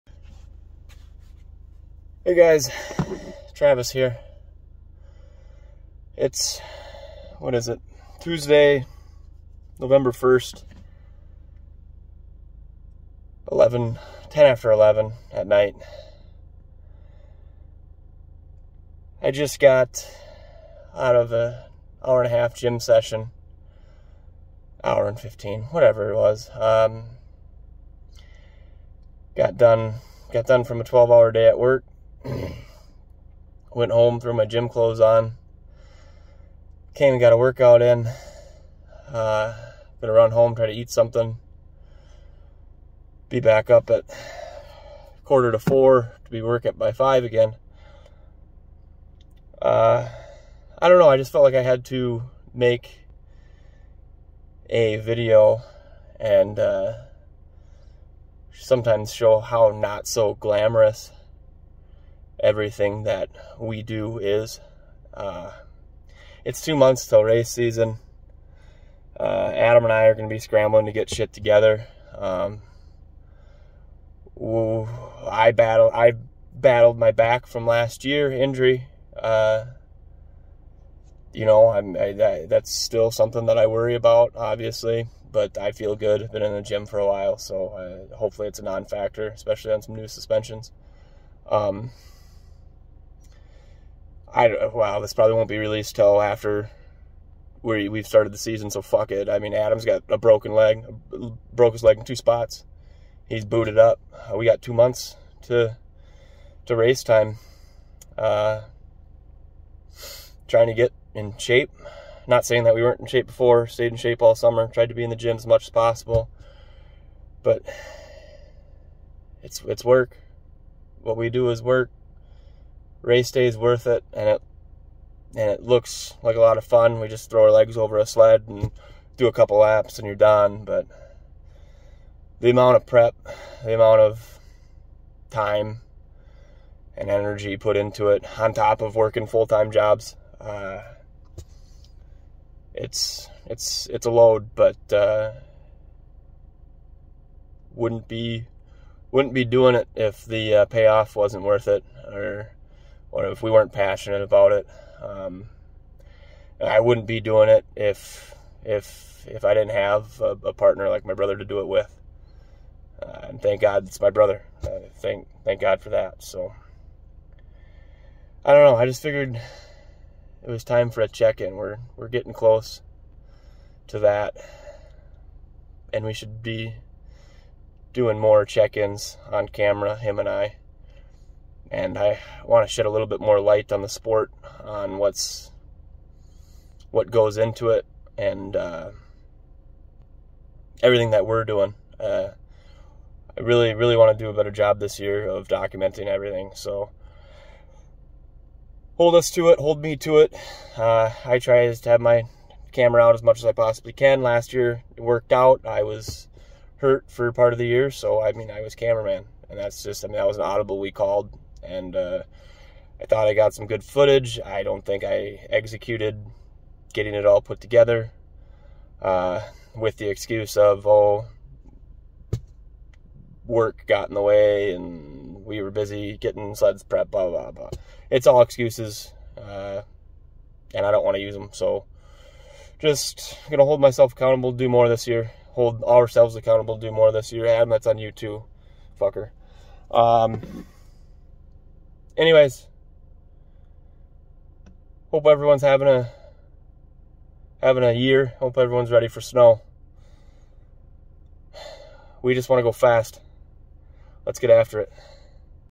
Travis Checks In!! 🤘🤘👢 11-1-22 Getting ready for racing
What all goes into cross country snowmobile racing???

Well. a lot. Its worth it. Don't take that wrong. But it is a lot of work and there is much that goes on behind the scenes that no one knows about. Full time jobs. Sleepless nights in the shop. Workouts well before sunrise or well after sunset. You name it. And we aren't the only ones. Everyone works hard.
This sport is something we care greatly about. The time we get to spend together with our family; the racing community that so quickly becomes family. WE ARE EXCITED and honored to be taking part of it.

This year will be especially exciting as we are trying something new. Plus, there are some hard fought battles happening right now, but we'll overcome and be ready on race day.

WE JUST WANNA GO FAST!!!